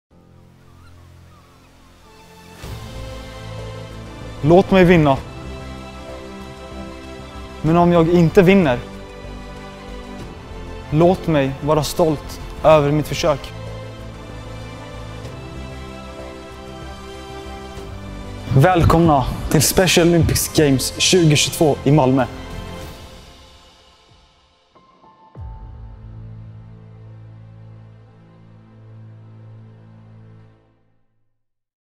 Teaser – Special Olympics Games Malmö 2022
Teaser inför Special Olympics Games som arrangeras i Malmö den 16-19 juni 2022.